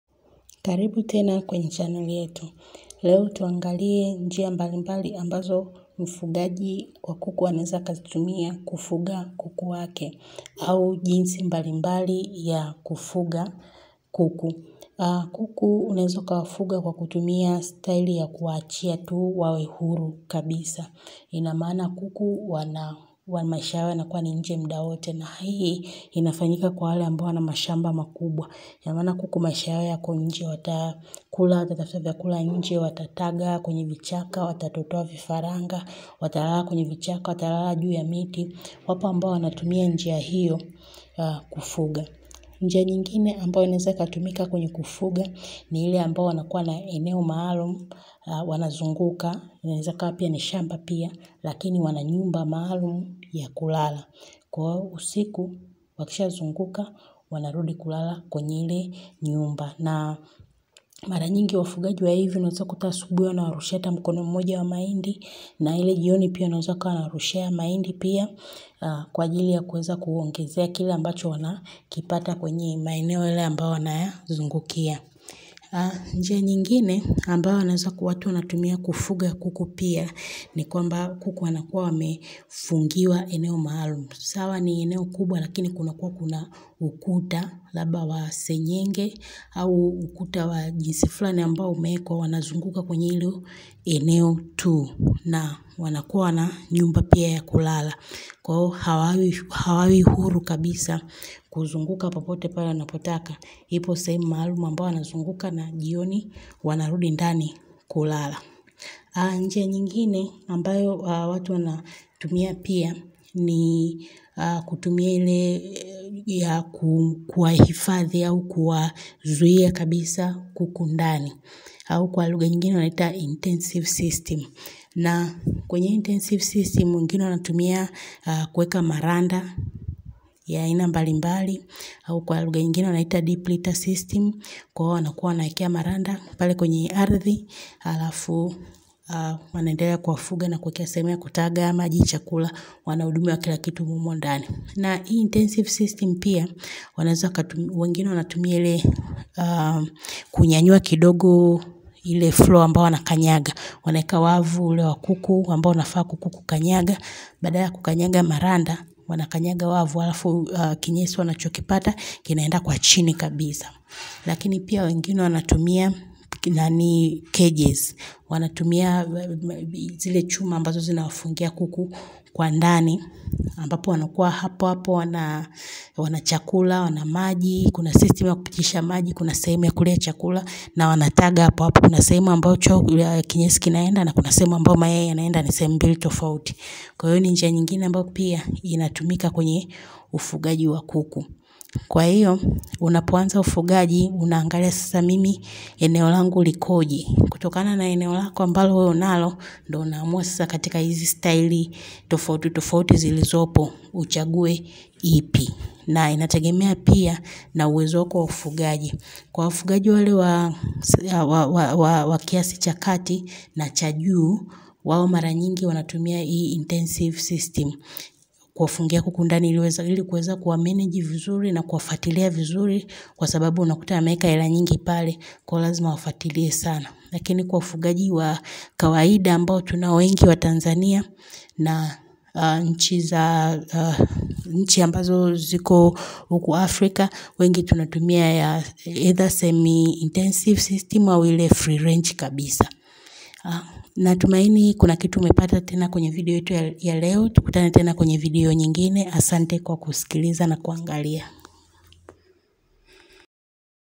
NJIA ZA KUFUGA KUKU
Kwenye video hii utajua njia zinazoweza kutumika katika ufugaji wa kuku katika mazingira tofauti tofauti.